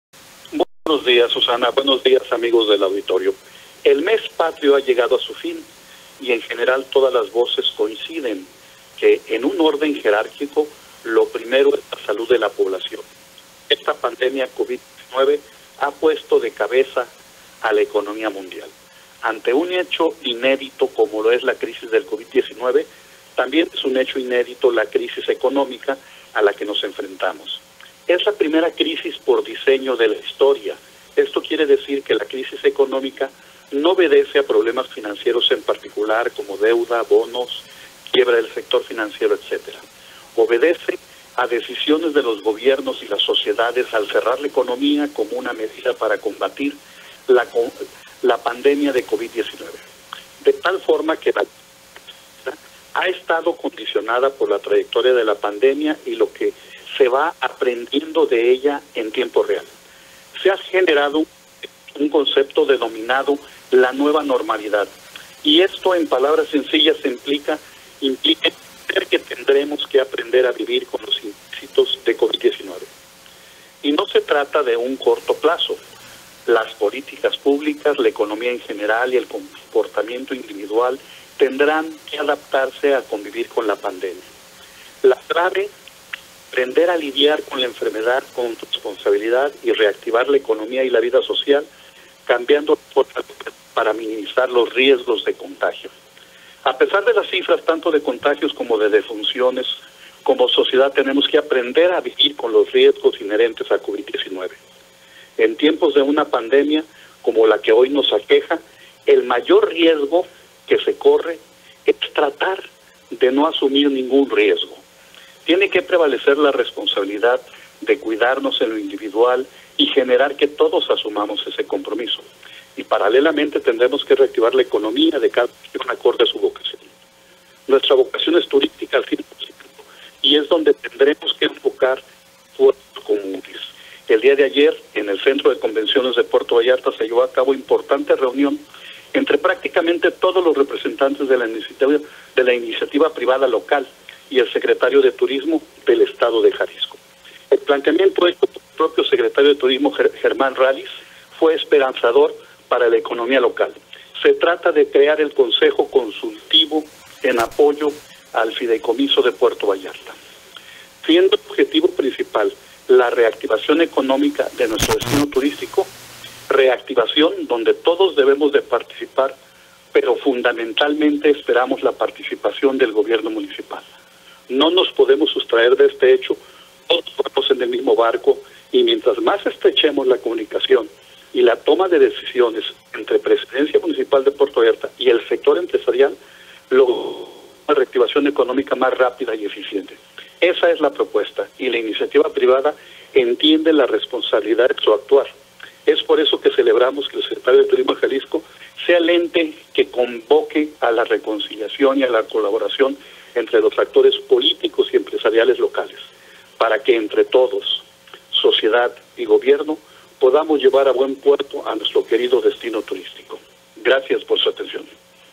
Jorge Villanueva nos da su opinión 29/09/2020
OPINIÓN DE: Dr. Jorge Villanueva Hernández, presidente de la Asociación de Empresarios de Puerto Vallarta y Bahía de Banderas